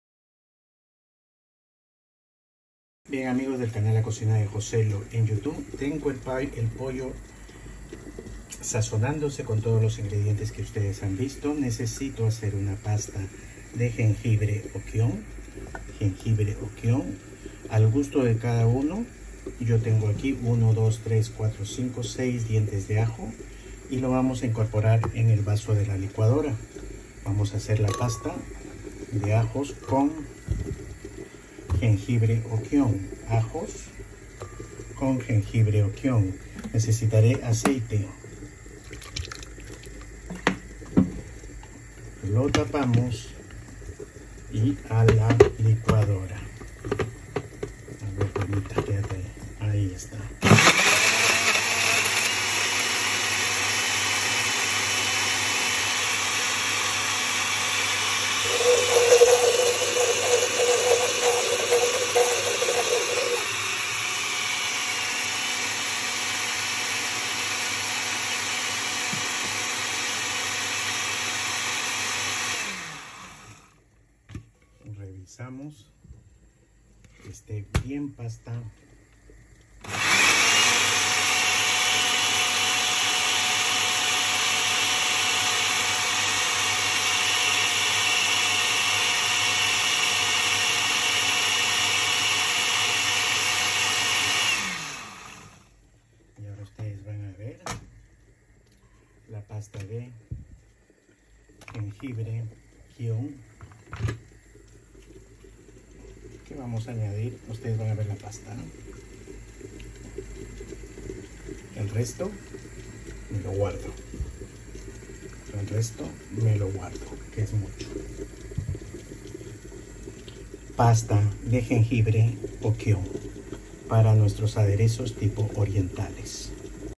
Pasta de ajos y jengibre o kion - La Cocina de Joselo
Lo suelo preparar para aderezar mis carnes o mis presas de pollo…